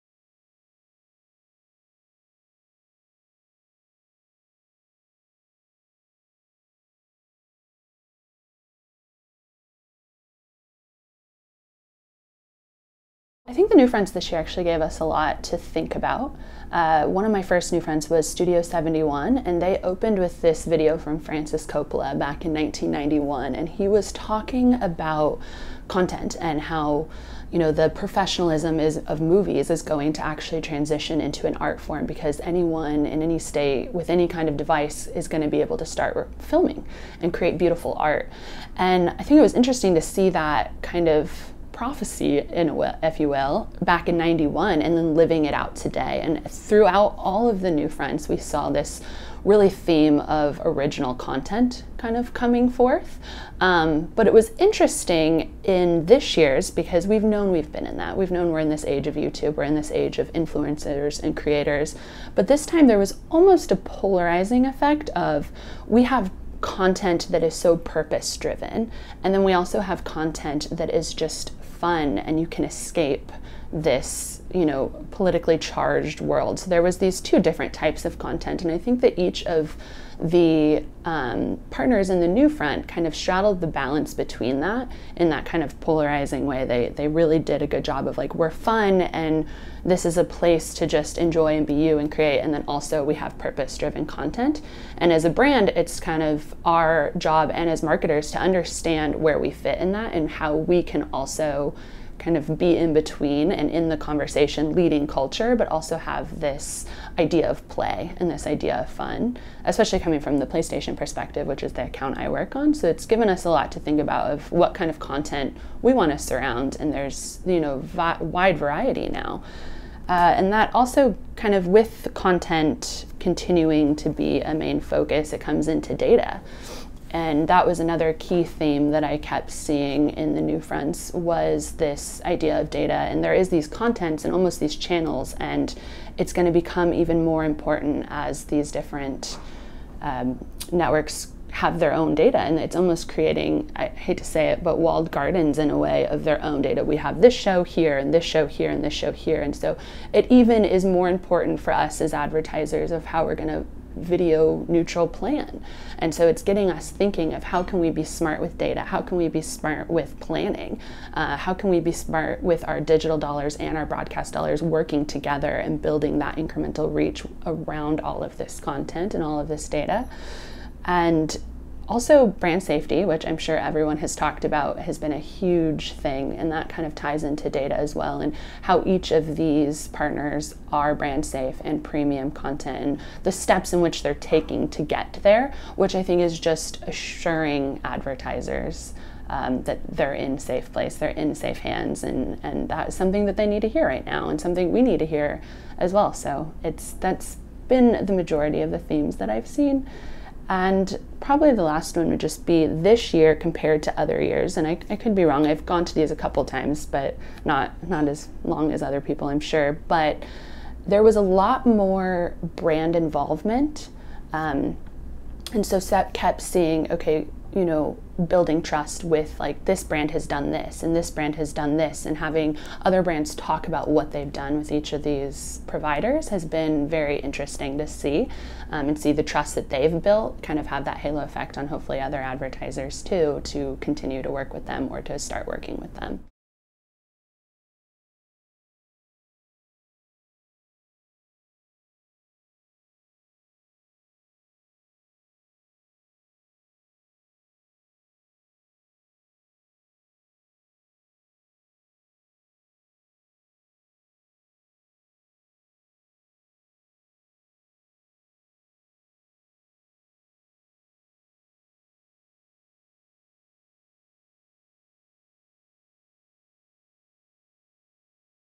MediaCom’s Capnerhurst on NewFronts’ Purpose
Is it a brand’s job to tap in to challenging and sometimes dark political times, or to help consumer escape the darkness?

Both, says one agency executive who was intrigued to see both opportunities presented to her at the recent NewFronts content shop.

In this video interview MediaCom digital investment associate director Heidi Capnerhurst says the content she saw, as premium publishers touted their upcoming slate to agencies and brands, ran the gamut.